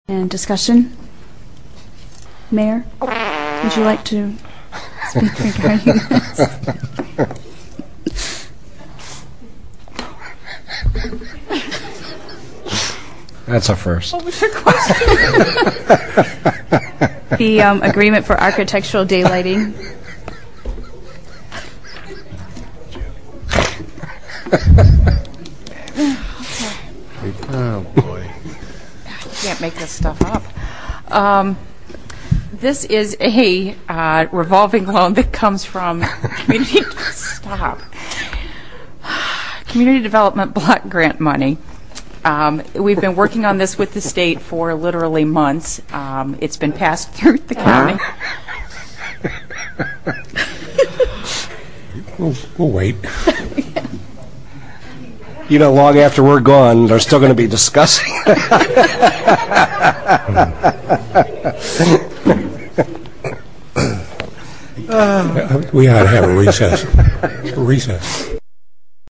Kid use fart machine during town counsil meeting
What a joke. Kid used fart machine during town counsil meeting.